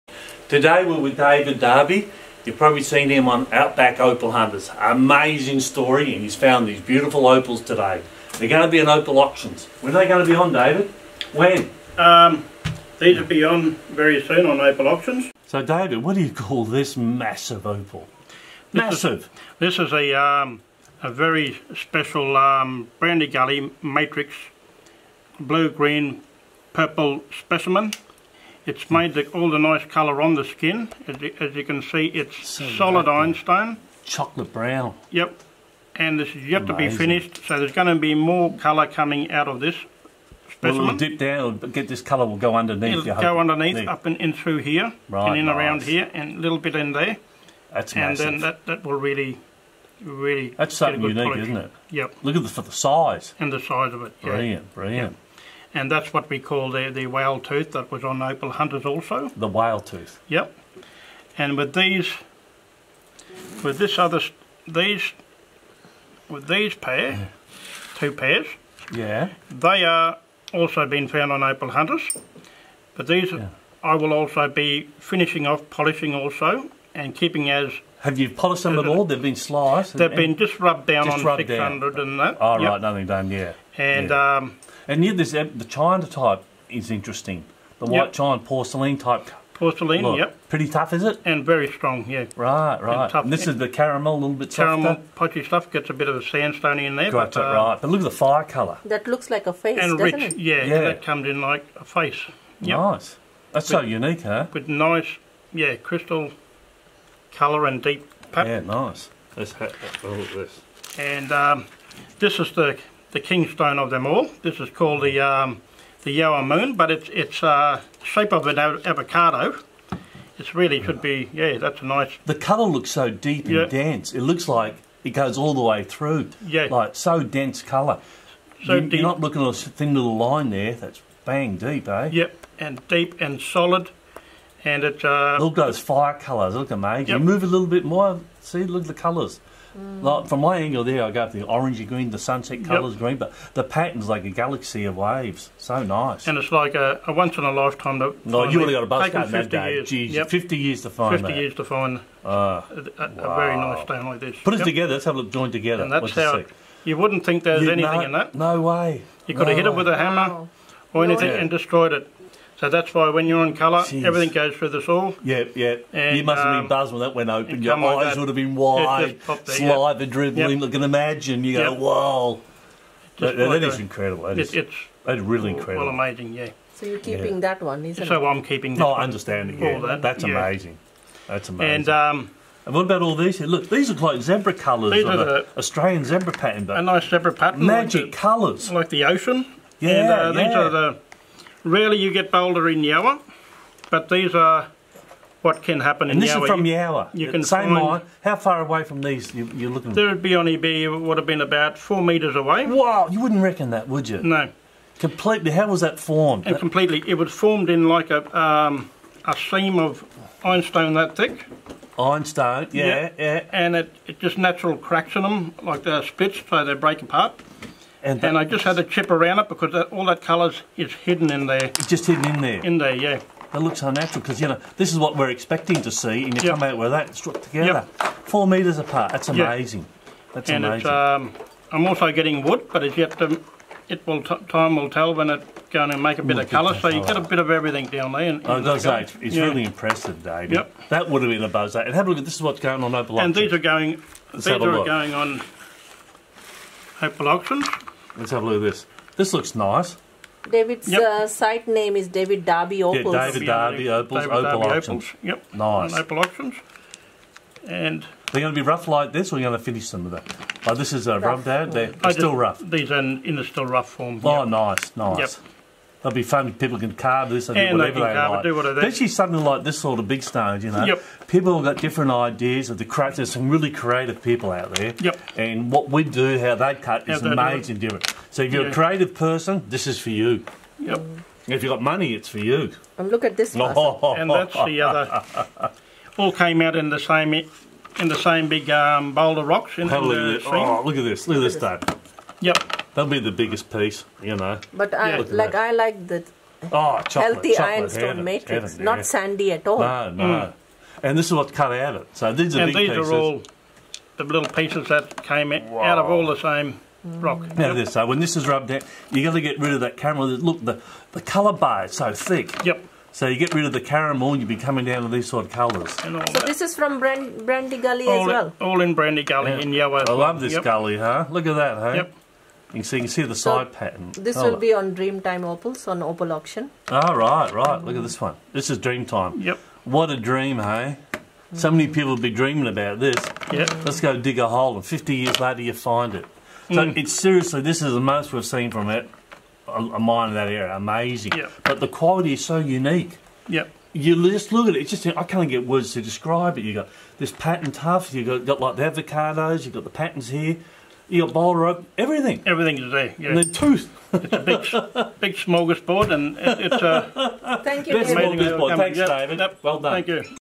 Outback Opal hunters million dollar find by David Darby in Yowah opal field! !
The parcel shown was found 8 meters away form the million dollar pocket, which was featured in the Outback Opal hunters discovery Channel show.
"Once in a lifetime find !" exclaimed David Darby who was featured on the Outback Opal hunters Discovery Channel show!
His incredible finds from Brandy Gully in Yowah will be for sale